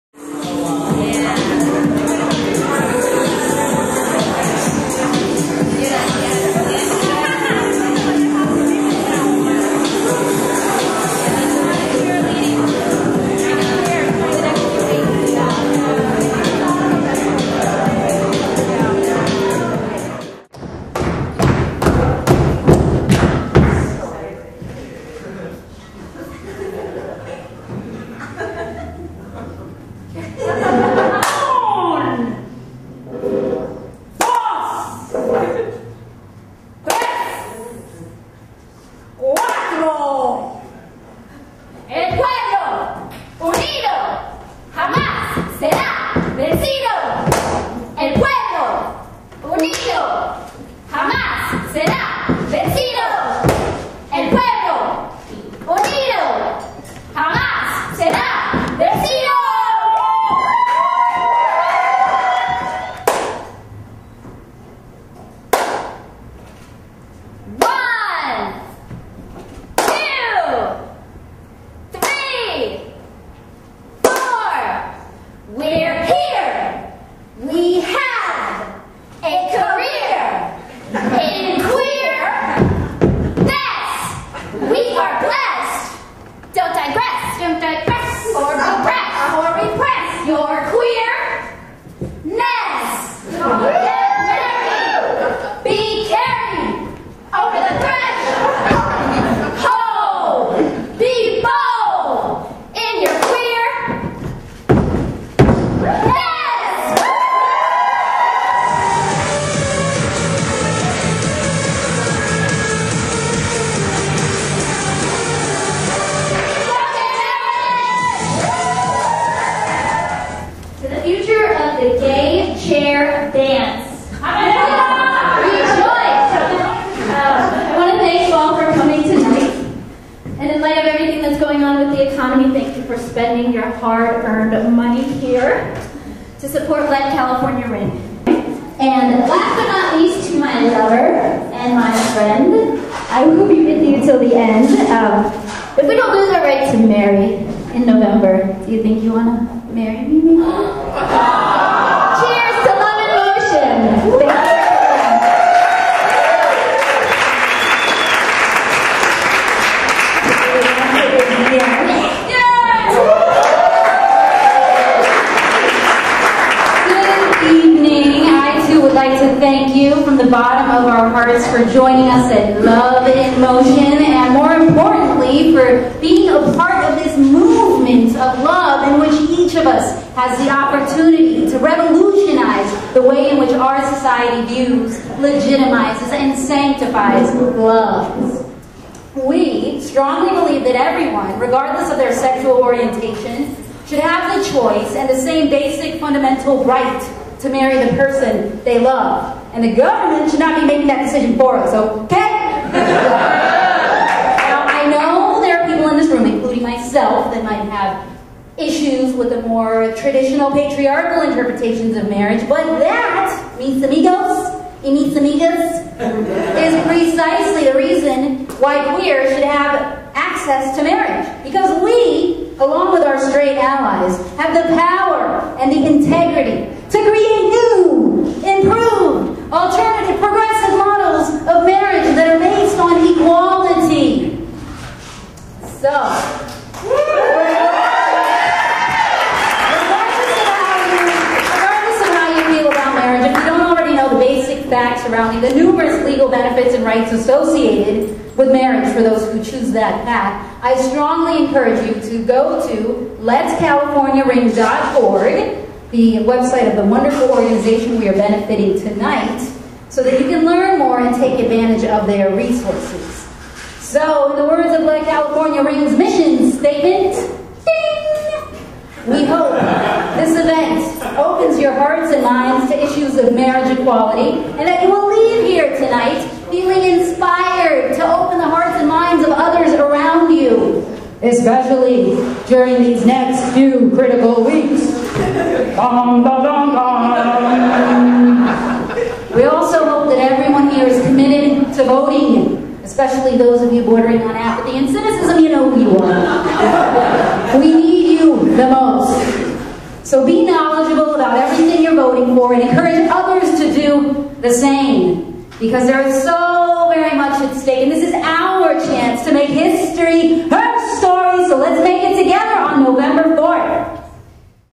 Queer Cheerleaders at Love in Motion
Director of Photography: Yun Suh

On October 4, Gigi Otalvaro-Hormillosa and her fiancee Heather Cox Carducci organized Love in Motion, a marriage equality fundraiser benefiting Let California Ring, a project of Equality California.  The event was co-sponsored by local companies such as 21st Amendment Brewery, Frey Winery, Blue Ice Vodka, Trader Joe's, Luscious Catering, Gold's Gym, and The Harrison Team at Sotheby's International Realty, among others.  The event drew approximately one hundred people throughout the night and featured entertainment by DJ Kinetic of Drift, Khalil Anthony of the Urban Folk Music Project, and host artists, Gigi and Heather, who performed their queer cheers and spoke about marriage equality awareness in the light of the upcoming election.  After the performance, Heather surprised Gigi and proposing to her on stage!